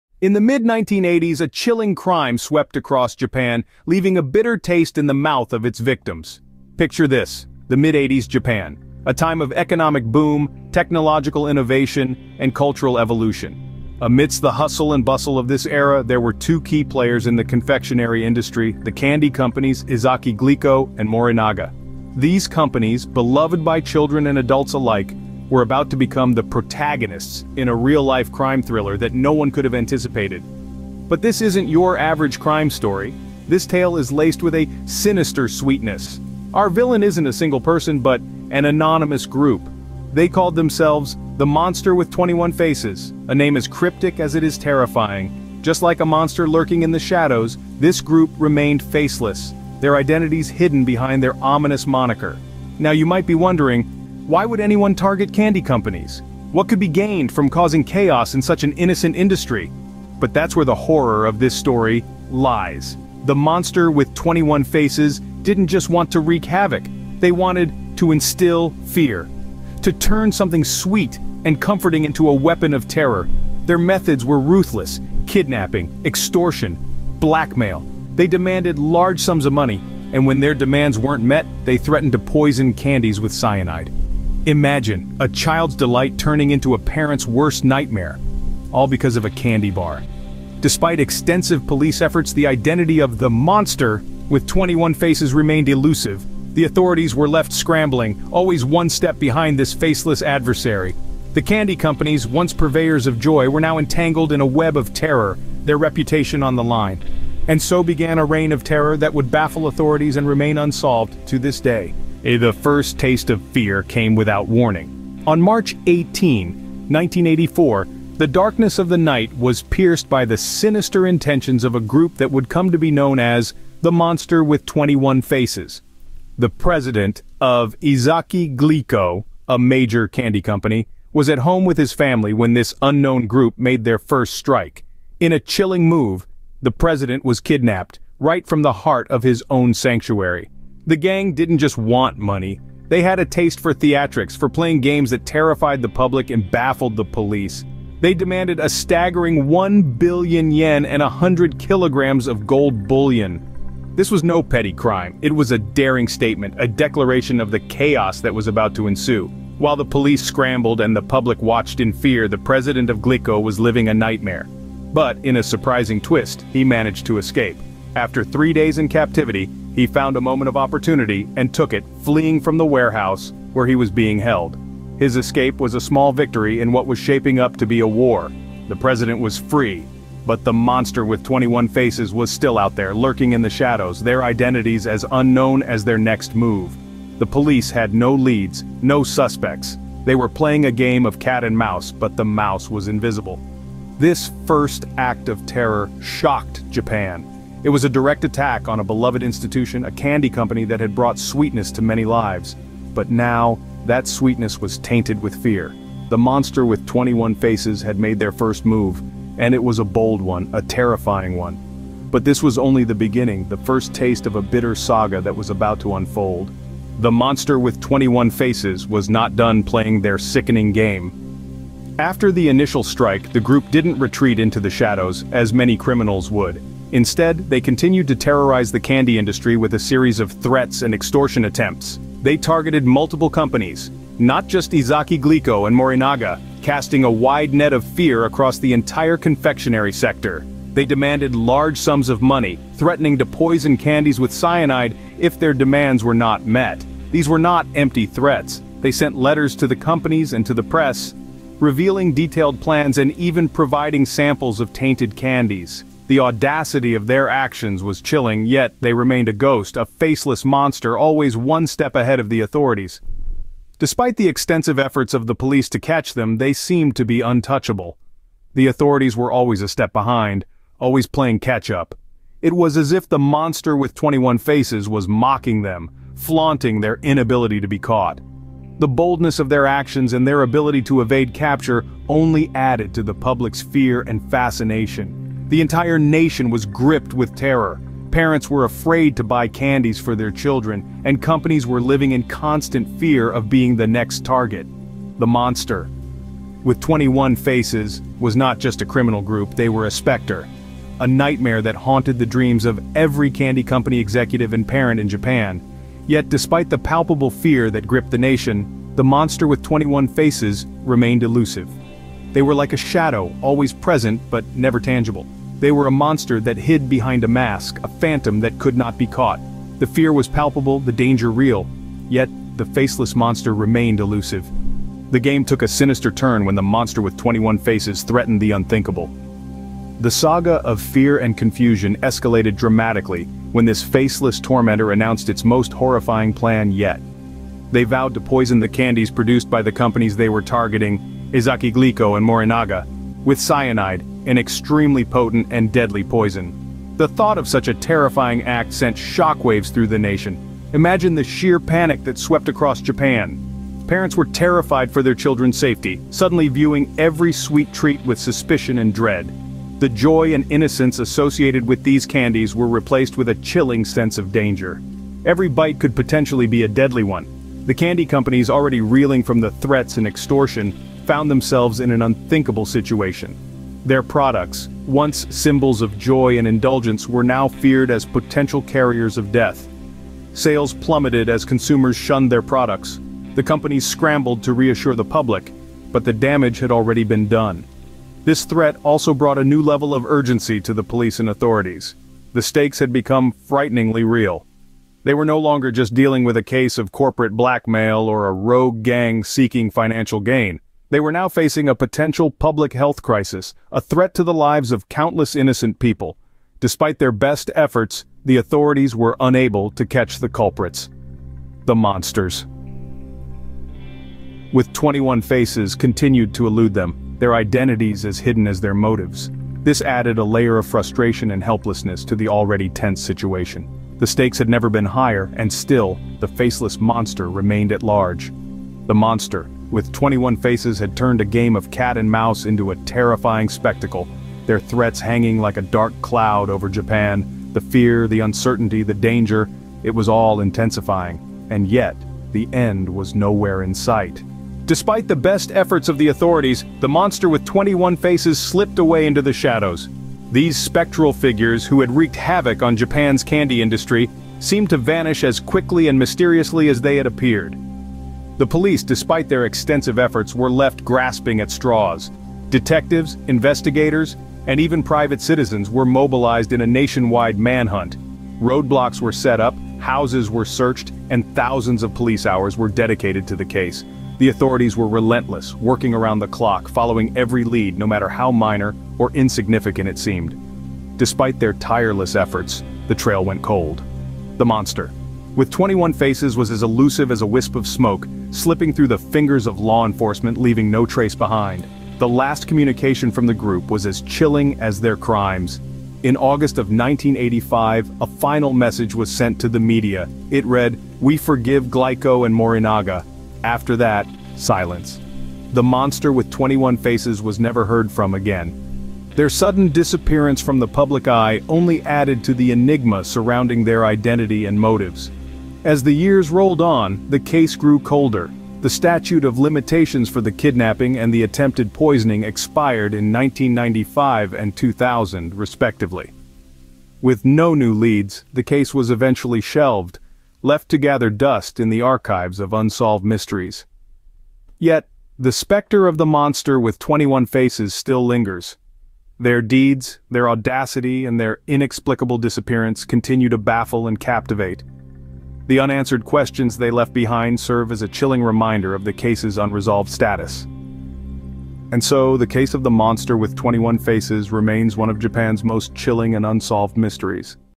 The Monster with 21 Faces - Mystery Of Japan’s Poisoned Candy
An unknown gang known as ‘The Monster With 21 Faces’ terrorized Japanese candy companies Ezaki Glico and Morinaga from March 1984 to August 1985. Employing aliases inspired by Edogawa Rampo's fictional villain, the ‘Monster’ orchestrated a series of crimes, including the kidnapping of Glico's president, Katsuhisa Ezaki. Security footage emerged of ‘The Videotaped Man’ believed to be the ringleader, putting allegedly poisoned Glico candy on store shelves in 1984. Letters, laden with defiance, were sent to police and the media, challenging authorities to apprehend them. Despite a relentless pursuit and a final ominous message, the ‘Monster with 21 Faces’ vanished, leaving a trail of unsolved mysteries and chilling uncertainties. No one has ever been apprehended.